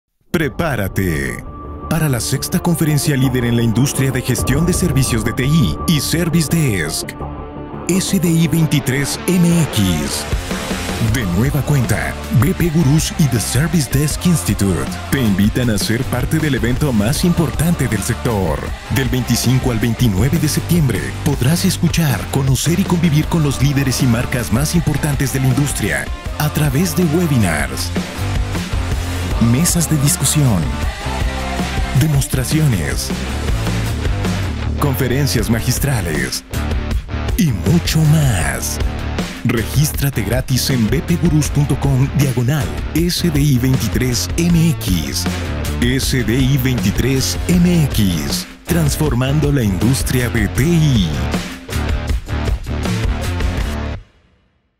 ¡Prepárate para SDI23MX!
Por sexto año, BP Gurus en alianza con @The Service Desk Institute te invitan a ser parte de SDI23MX; un foro para las tendencias y retos que enfrentan los sectores de ITSM, Service Desk, TecnologíasDelFuturo y TransformaciónDigital.